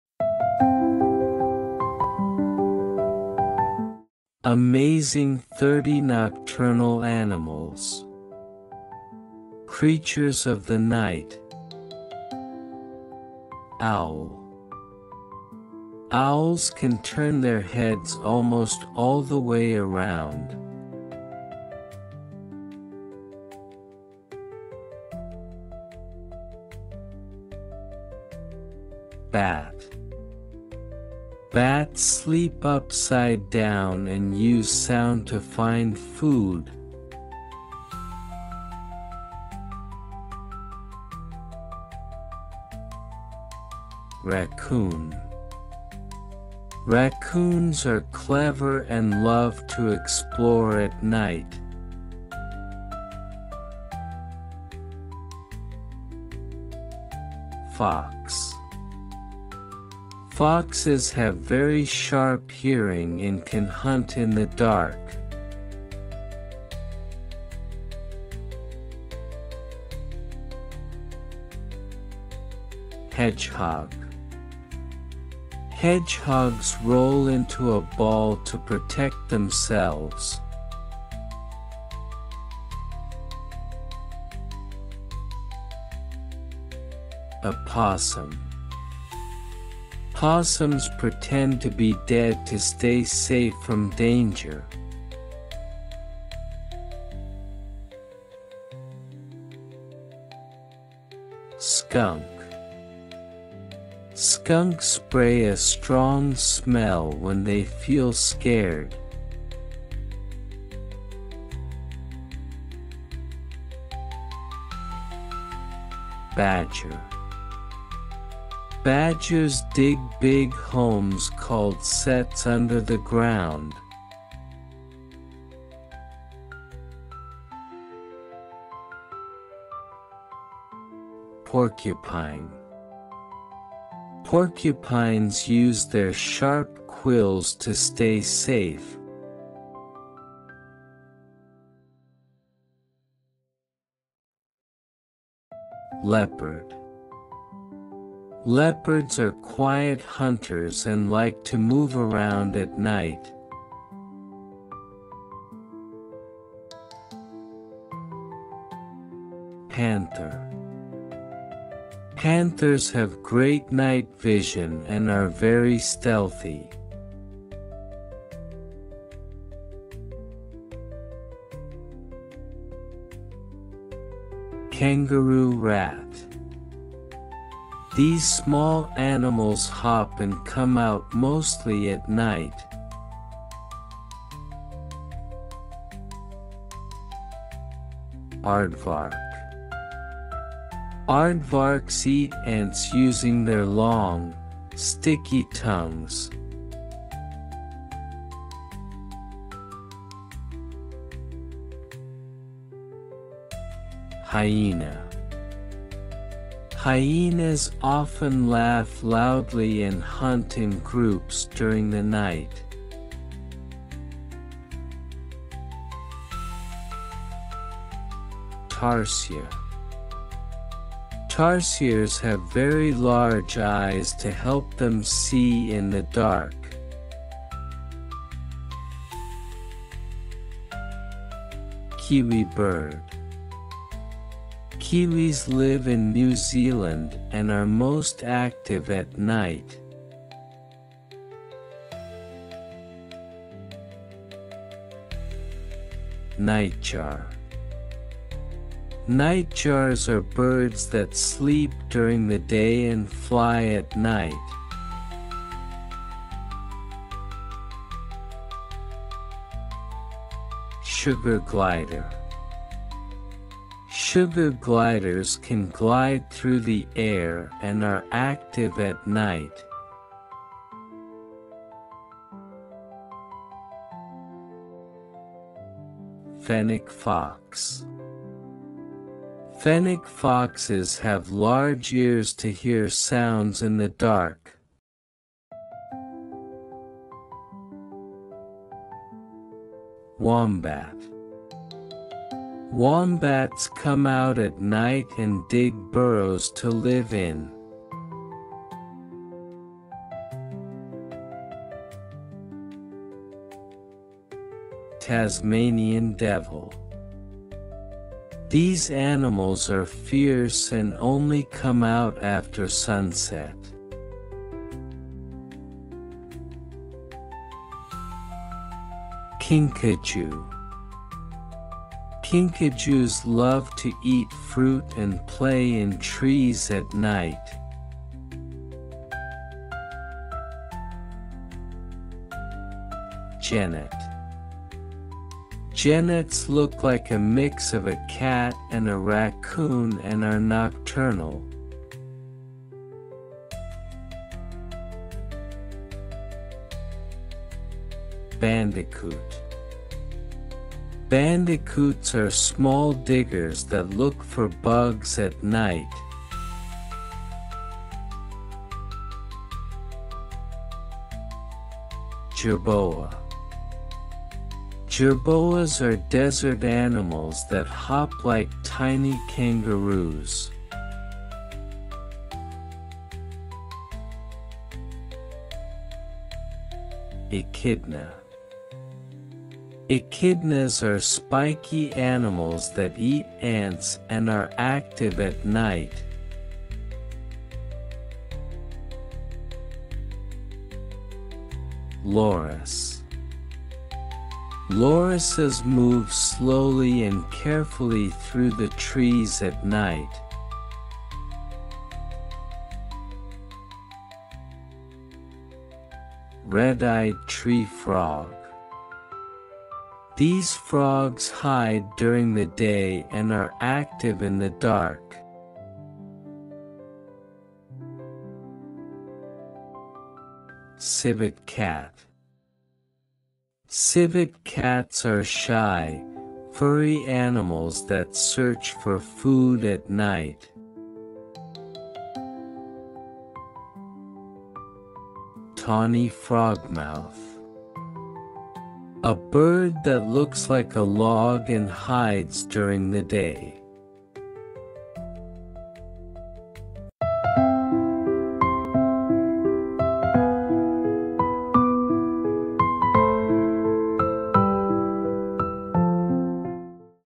30 Nocturnal Animals for Kids | Fun Facts About Creatures of the Night | Tiny Tutors Learning
30 Nocturnal Animals for Kids | Fun Facts About Creatures of the Night | Tiny Tutors Learning

🌙 Welcome to Tiny Tutors!
Hello curious little explorers! 🦉
Today, we’re discovering the magical world of Nocturnal Animals—those special creatures that come out when the sun goes down. From owls to bats and kangaroo rats to civet cats, let’s learn 30 amazing nighttime animals and fun facts just for kids!

🐾 What’s Inside This Video:

30 Nocturnal Animals from around the world

Kid-friendly facts and fun emojis

Perfect for Cambridge Kindergarten (UKG, LKG) and preschool learners

🎉 Learning made fun with animals that roam under the stars! 🌟

🎨 Follow Us for More Fun & Learning!